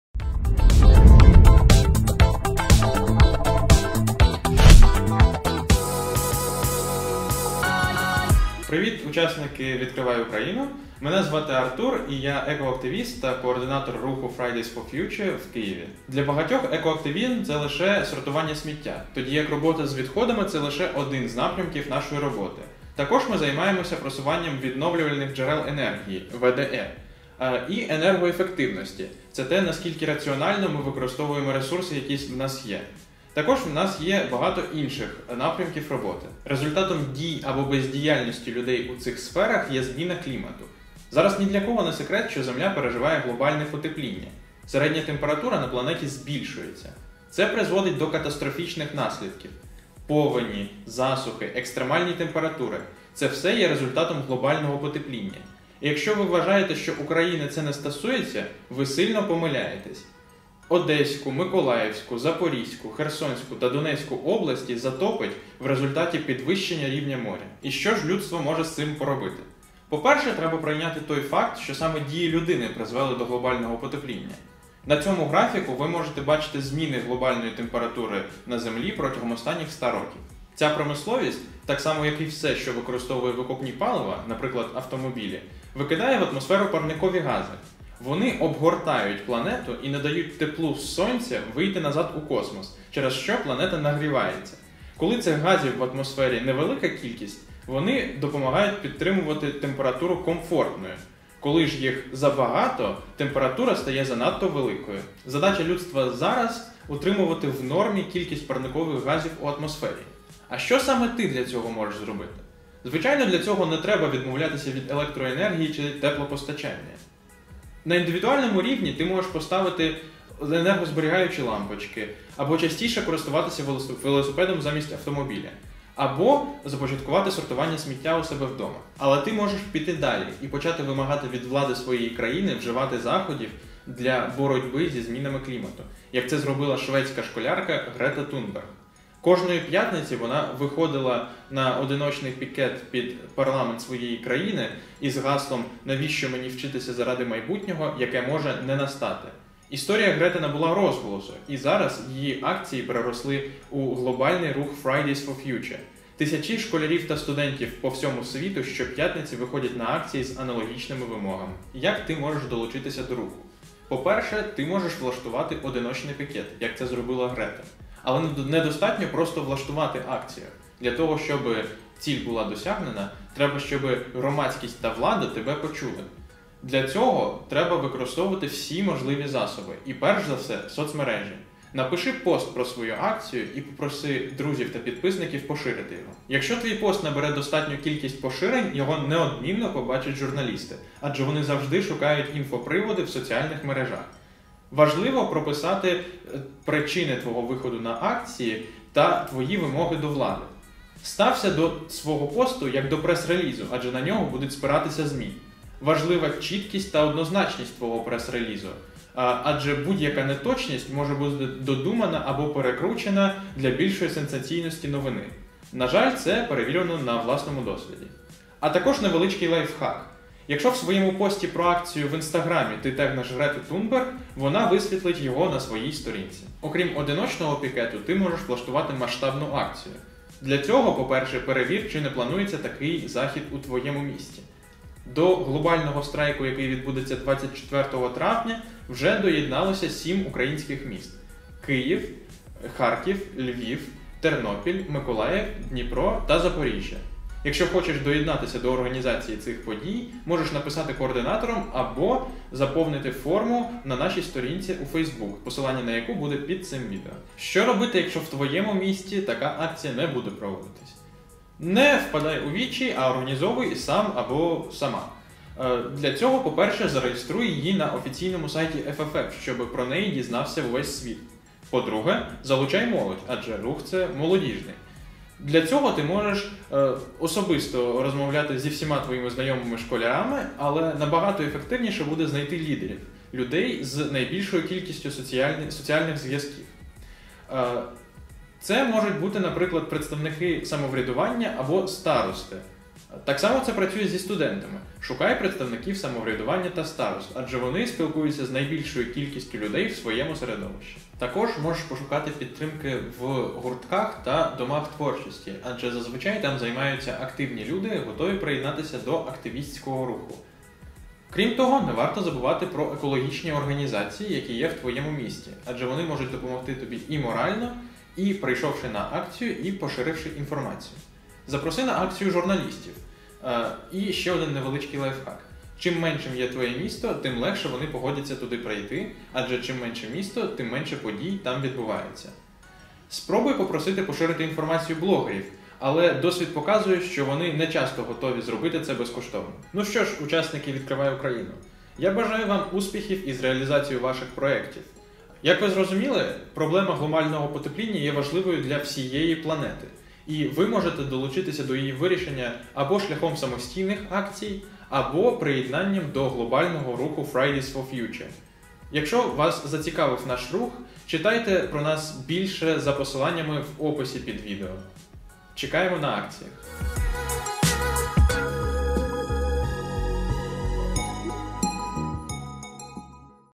Відкривай Україну Лекція - Екоактивізм
Лектор Артур Саркісян - Координатор Fridays for Future у м. Києв. розповість, що  таке екоактивізм та напрямки його діяльності. Наслідки глобального потепління для України та світу. Як вплинути на глобальне потепління на індивідуальному і на глобальному рівні. Та декілька практичних порад, по комунікації своїх вимог та позиції.